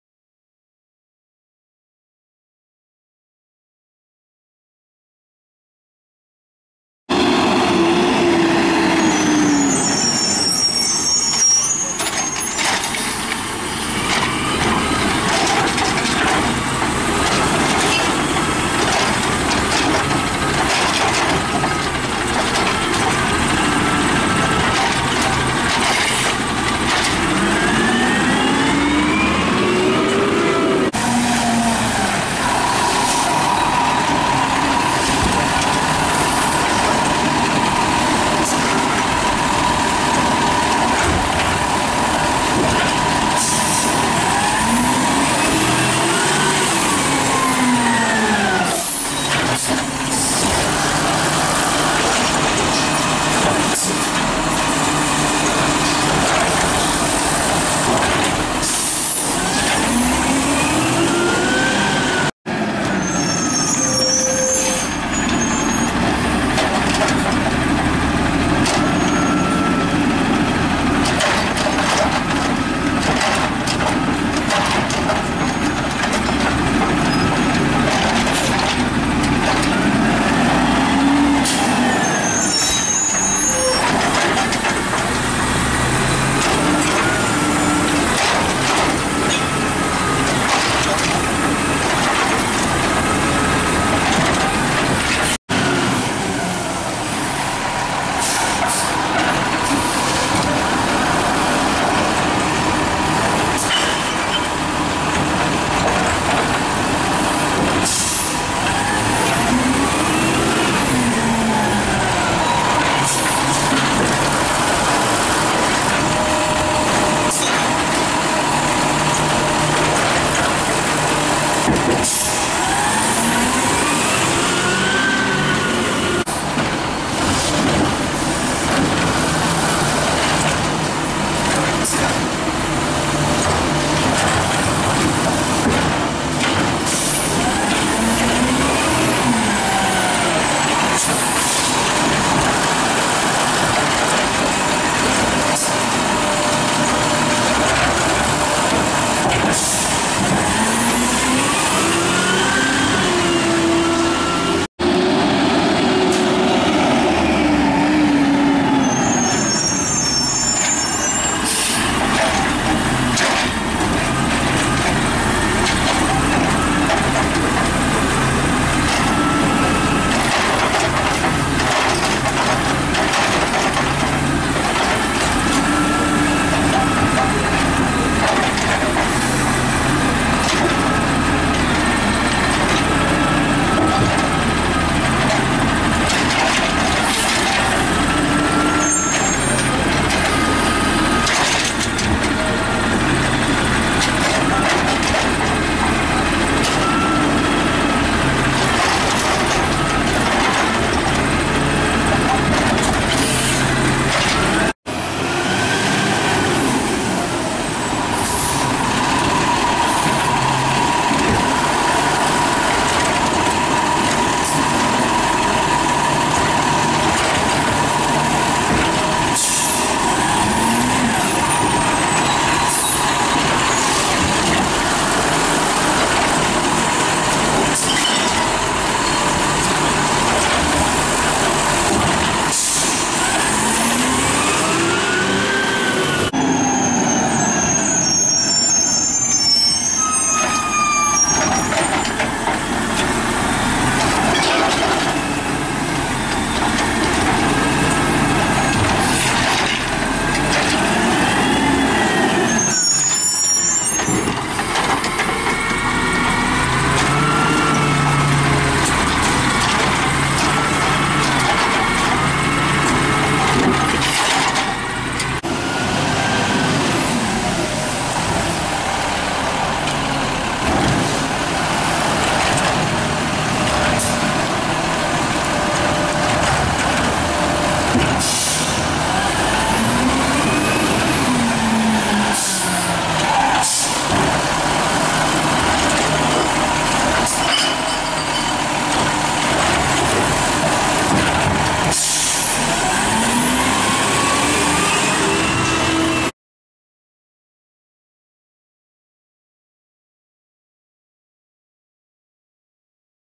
Scorpion VS Amrep Garbage Trucks (Eps. 1)
VS Episode 1: Here we have 2 different Peterbilt 320 garbage trucks.  The first is a mid 2010s CNG Scorpion in Scottsdale, and the second is a mid 2000s refurbished diesel Amrep with a new body and arm.
Which truck do you think runs better?
Do you think the weather can effect the trucks' performance?